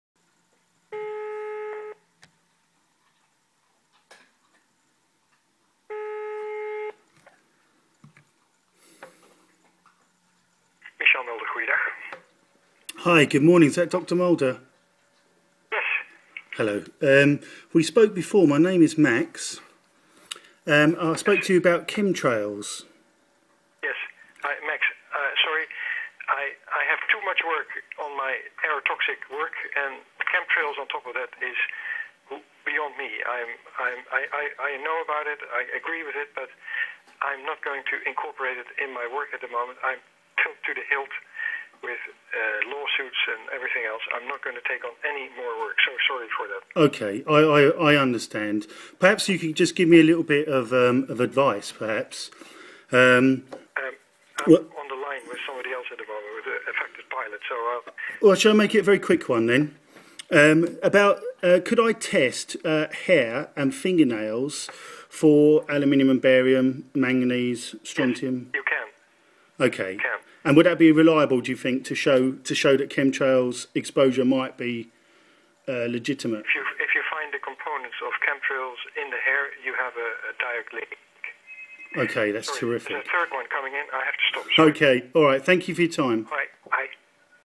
Aerotoxic Doctor Chemtrails Confirmation Hair & Nails
Mirrored from MrMaxBliss please drop him a sub and check out the links, he is working for our freedom
A brief call to the Doctor leading the investigation into Aerotoxic Syndrome. Not many know of this issue that is becoming more acknowledged . A very busy man as many pilots and frequent flyers are becoming chronically ill and sadly some have even died.

MrMaxBliss;
I have talked at length with this Doctor about chemtrails. He has confided that they have been spraying for many years. I suggested there might be a link between the chemtrail atmosphere and cabin air as the planes draw the air from outside through the engine via a bleed valve. He stated he was looking for a specific exotic ingredient to bring down the airline companies on behalf of his many suffering clients. Dr Mulder is also a former pilot for KLM and it is reassuring to hear pilots acknowledging the chemtrail program. He had stated he would test for alu and barium but with a follow up call to him, he confirmed he was too busy and the clients pay for the expensive tests.

If the chemtrail program is happening, then the many planes are constantly flying through concentrated toxins, which have repeatedly been found in hundreds of rain water analysis around the world. What a pity Dr Mulder is so busy, I was lucky to speak with him because maybe this line of investigation really could expose the toxic dangers that Neurologist Dr Baylock reveals about chemtrails and the detrimental effects on the brain.

WE ARE APPEALING TO FREQUENT FLYERS TO CONTACT US AND VOLUNTEER A HAIR OR NAIL SAMPLE... (anonymity) To establish the levels of toxins you may exposed to and to add more conclusive data to bolster the evidence and expose the likely chemtrail or geoengineering program.